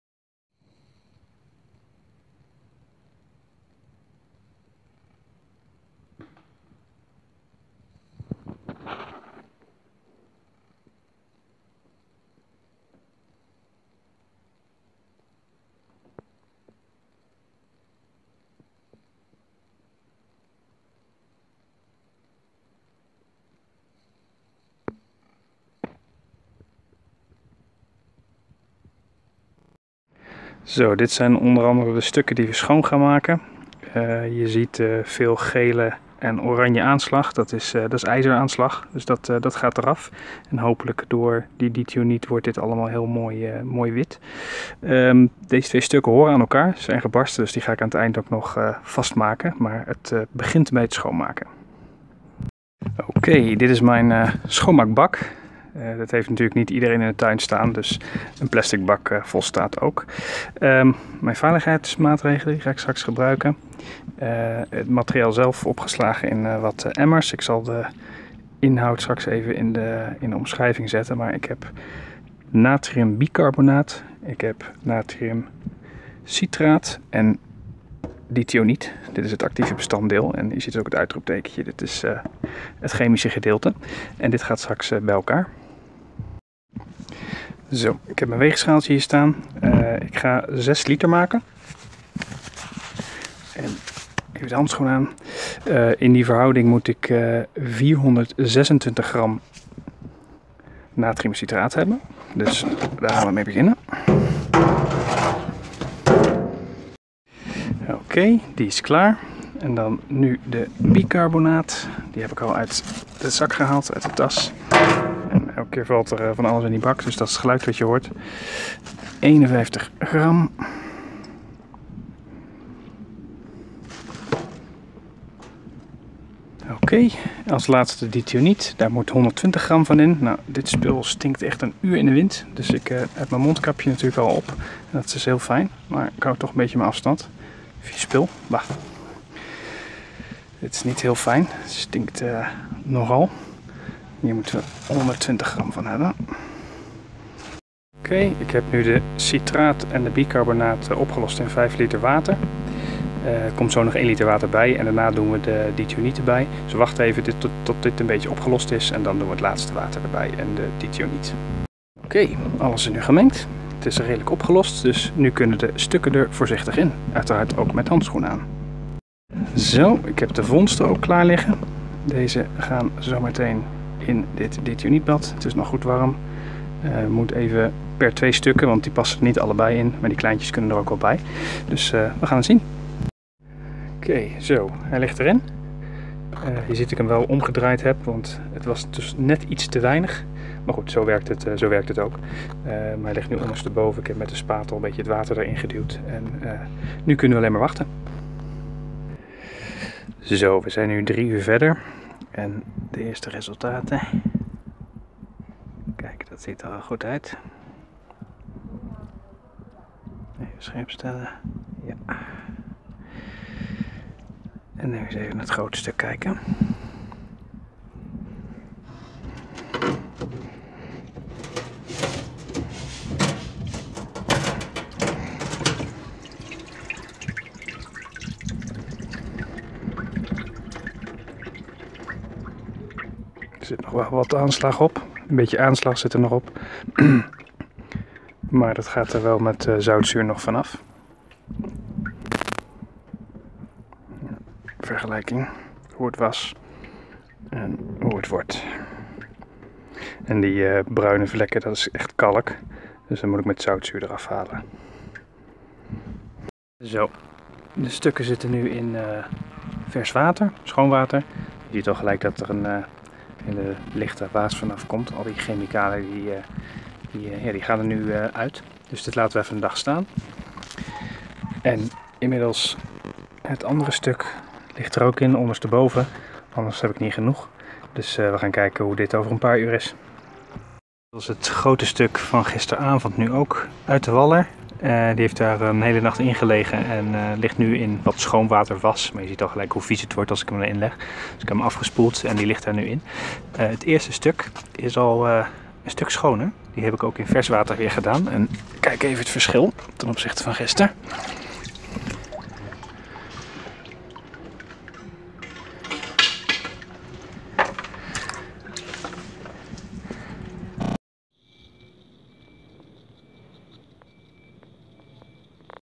Kristallen edelstenen mineralen schoonmaken met natrium dithioniet
Hoe kan je kristallen edelstenen mineralen schoonmaken?. In deze video laten Daniel Middel en Kristian Egberts zien hoe je zelf gevonden kristallen, in dit geval kwarts en bergkristal, kan ontdoen van ijzeraanslag of roest. Hiervoor gebruiken zij een combinatie van enkele chemische stoffen, bekend als dithioniet. Online is hiervan een kant-en-klare variant beschikbaar onder namen als Mineral Clean en Super Iron Out.

Voor verhoudingen en oplossen in de juiste volgorde van deze stoffen, zie dit vrij vertaalde document (niet door ons opgesteld)

Waarschuwing: Het gebruik van chemicaliën is gevaarlijk! Gebruik altijd beschermende kleding en hanteer de nodige verplichte voorzorgs- en veiligheidsmaatregelen. Werk altijd buitenshuis met voldoende ventilatie. Voer chemische stoffen af naar het daarvoor beschikbare afvalstation. Deze video is gemaakt door professionals; het gebruik van deze chemicaliën en volgen van deze tips is volledig op eigen risico; wij zijn op geen enkele wijze verantwoordelijk voor schade en gevolgen van het zien van deze video en zelf toegebrachte schade en/of letsel door onjuist gebruik van soortgelijke chemicaliën of verkeerd uitvoeren van deze handelingen.